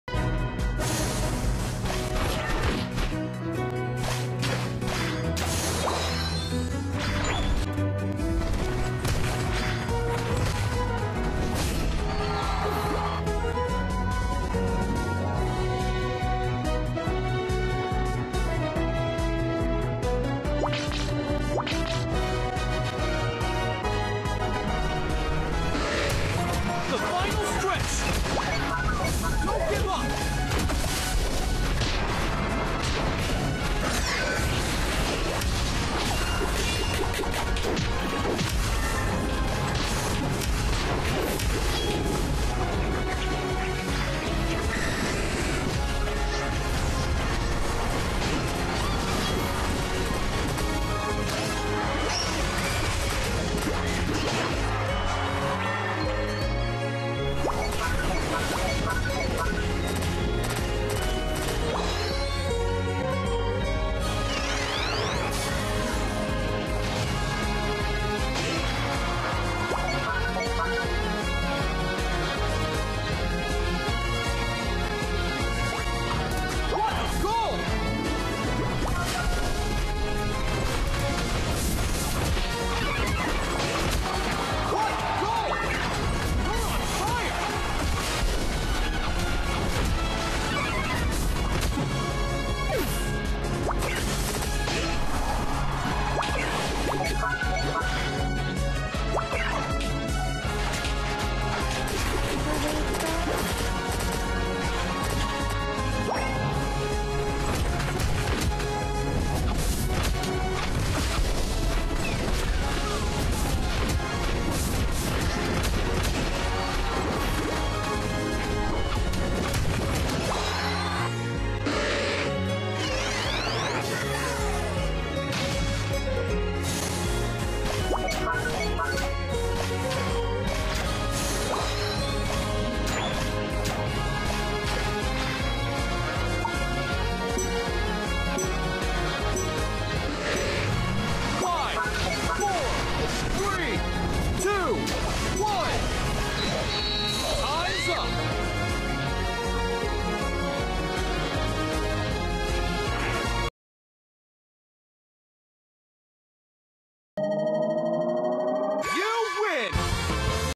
No Tyranitar allowed
I will not let it live!

Slowbro Set:
Focus Band (Level 30)
Choice Specs (Level 30)
Shell Bell (Level 30)

Emblem effects:
Hp: +60
Atk: -8.5
Def: +4

Bonus effects:
+8% Cooldown Reduction